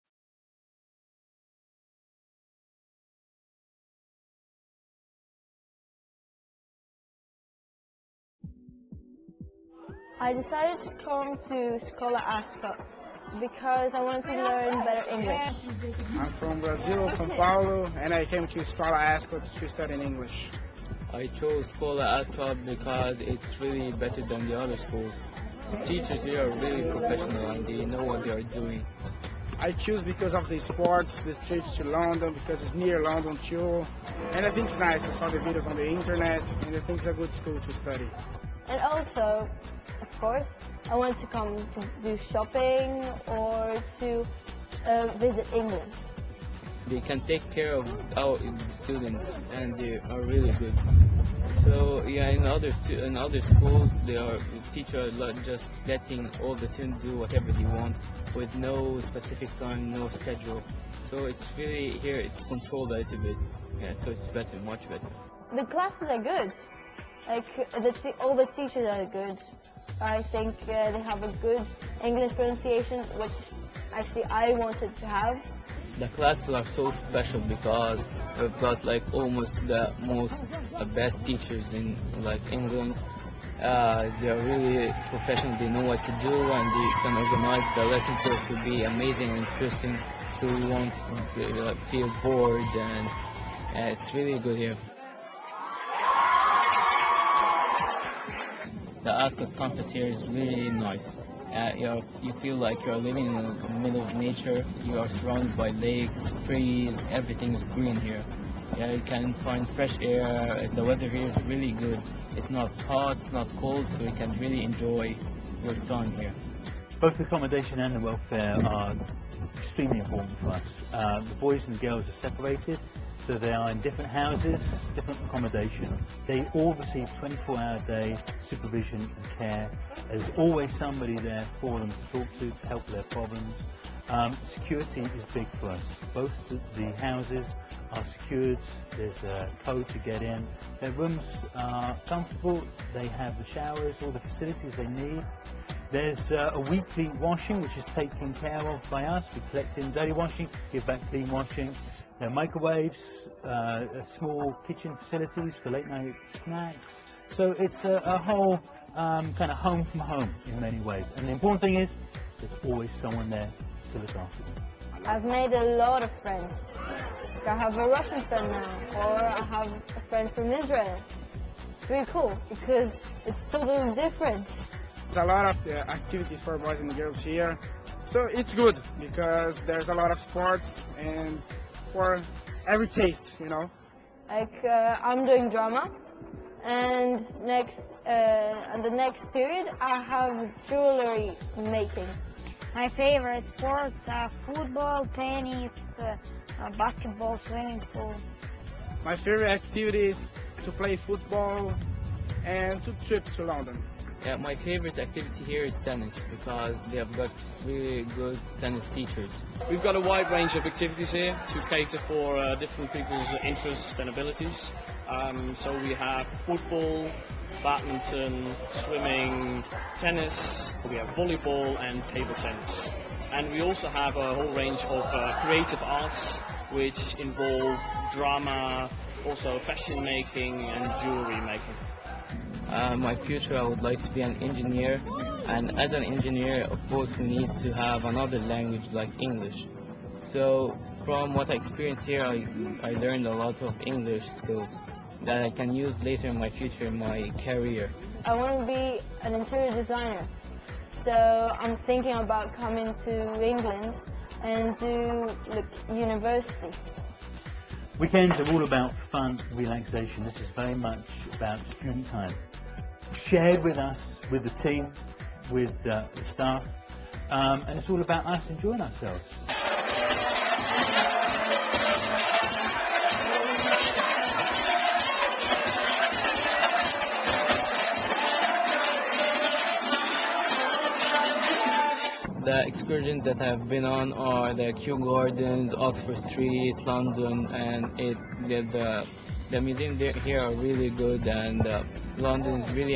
SKOLA Ascot Summer School
SKOLA Ascot Summer School is a junior English course with 15 hours of English language and 10 hours of our Discover London excursion programme.